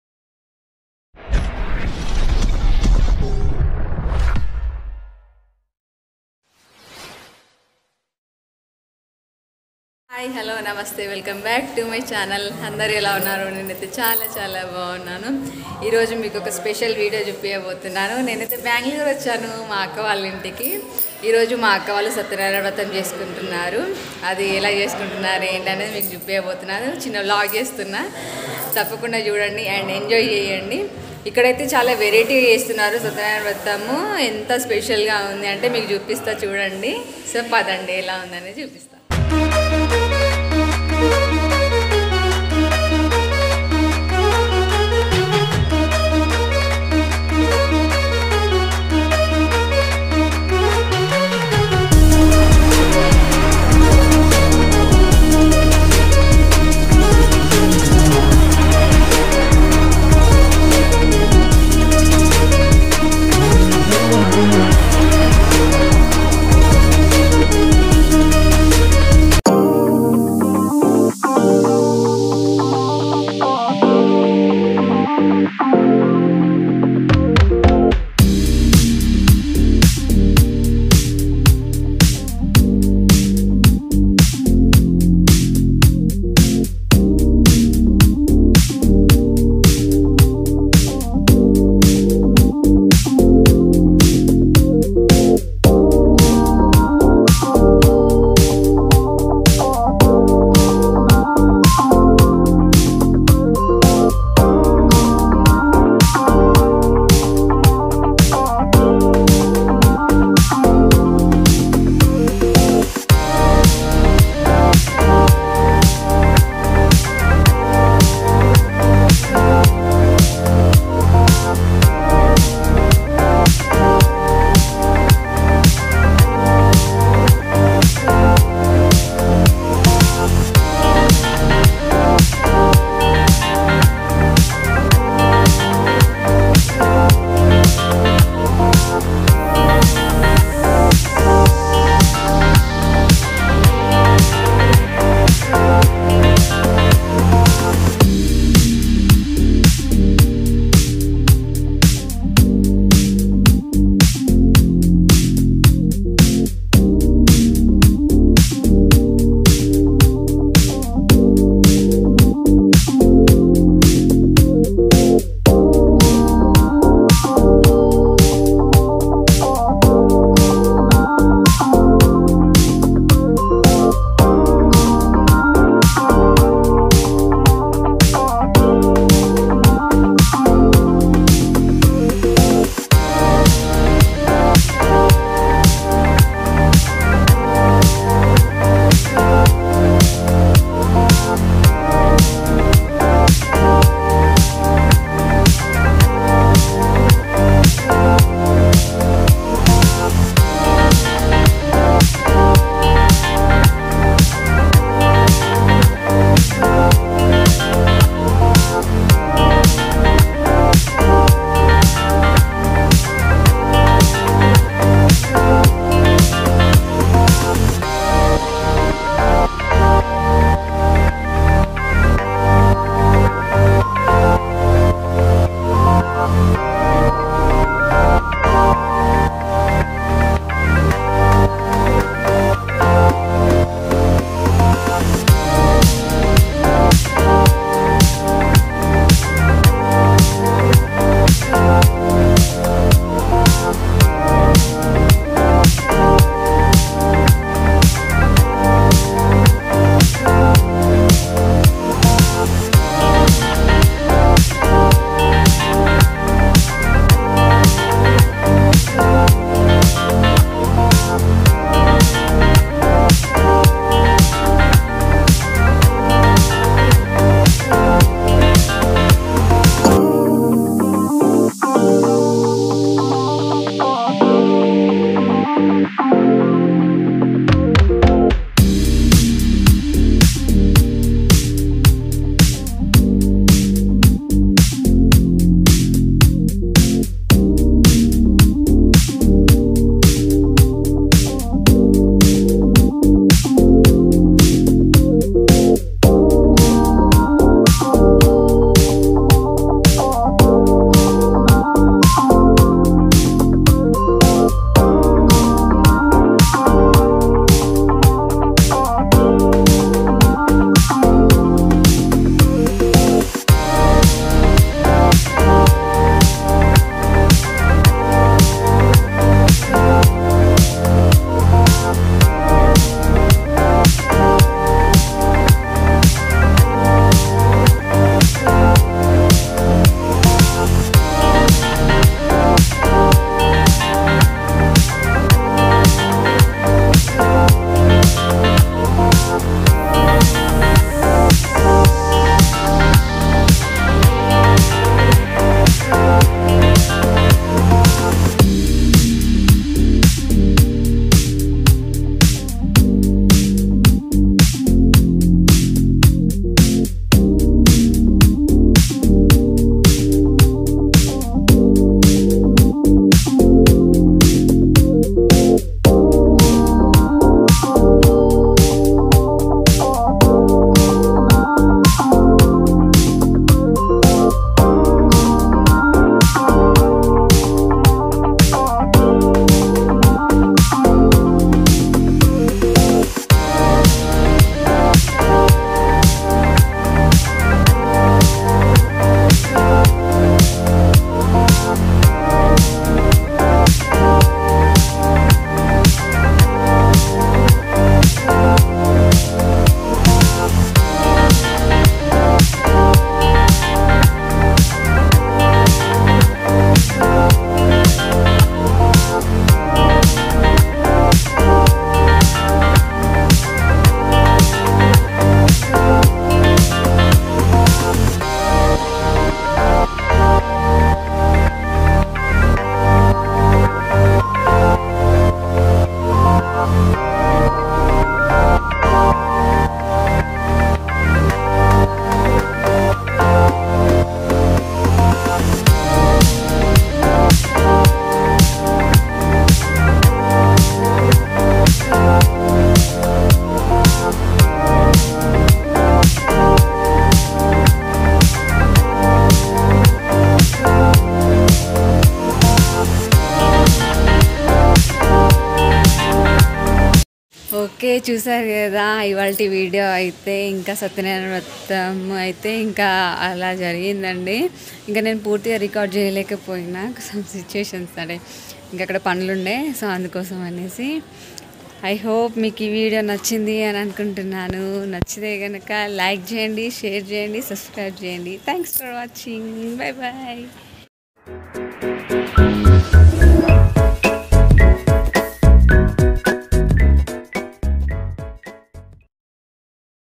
Satyanarayana Swamy Pooja Vratham Bangalore | సత్యనారాయణ స్వామి పూజ వ్రతం
Hello Everyone
In this video we covered a vlog
Satyanarayana swamy Pooja / vratham in Bangalore